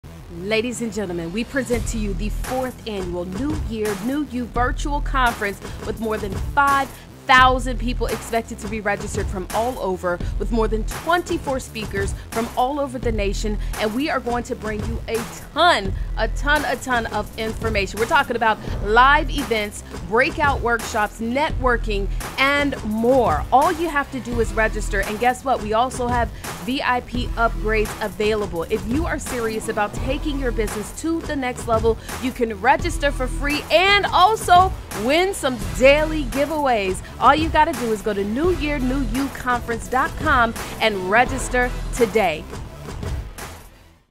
New Year New You Virtual Conference 2021
Have you registered for the New Year New You Virtual Conference?
This is not like any conference you have been to.

We promise its not just about being motivated but we also plan to give you tangible steps of things you can do to get started on your journey to success.

VIDEO GEAR, LIGHTING USED TO SHOOT THIS VIDEO:
Camera - Canon EOS Rebel T5i EF-S 18-55 IS STM Kit by Canon

Light - LimoStudio 18" Ring Light